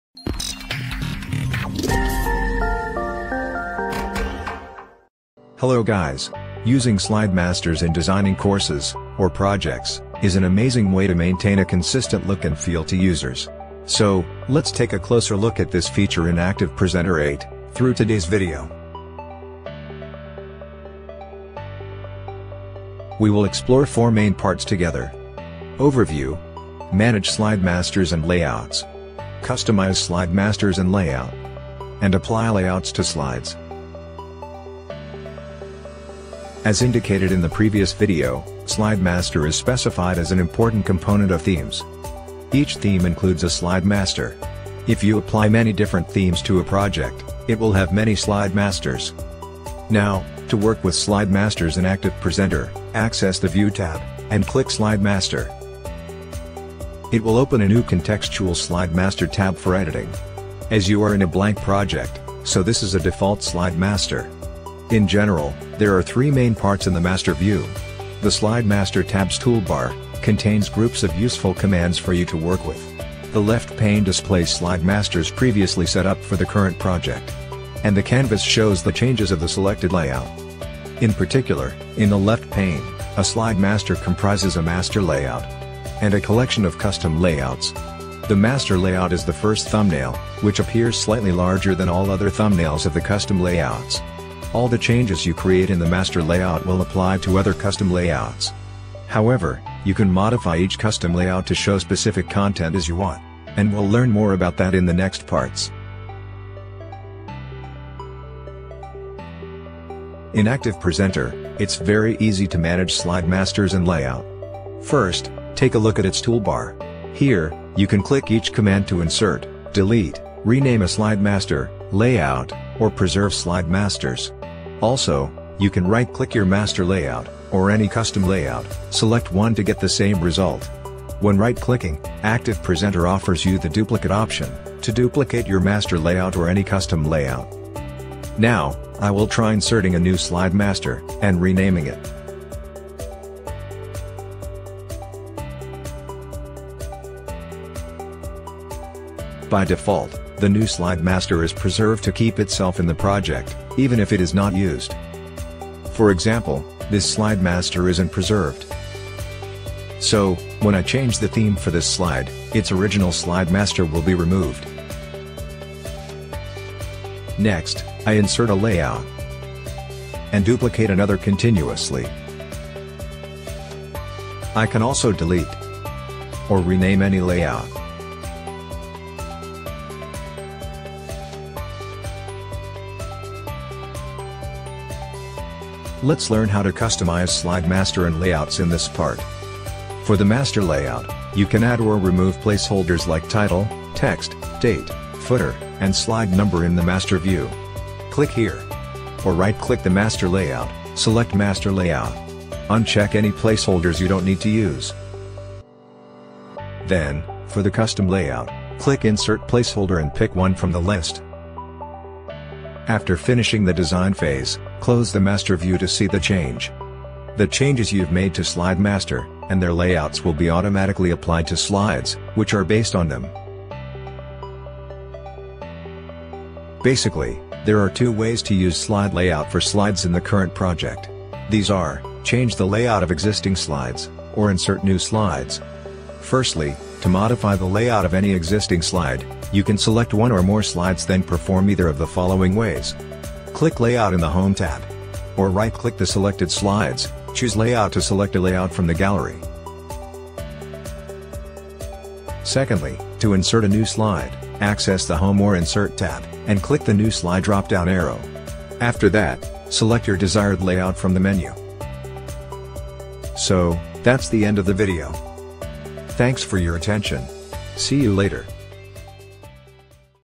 How to Use Slide Masters - ActivePresenter 8
Using Slide Masters in designing courses or projects is a great way to maintain a consistent look and feel for users. Thanks to Slide Masters in ActivePresenter 8, you can easily create the default themes, colors, fonts, preset background styles, text, objects and determine the object placement in slides.
So, let’s explore how amazing this feature is in today’s video.
0:33 - Overview
1:47 - Manage Slide Masters & Layouts
3:03 - Customize Slide Masters & Layouts
3:43 - Apply Layouts to Slides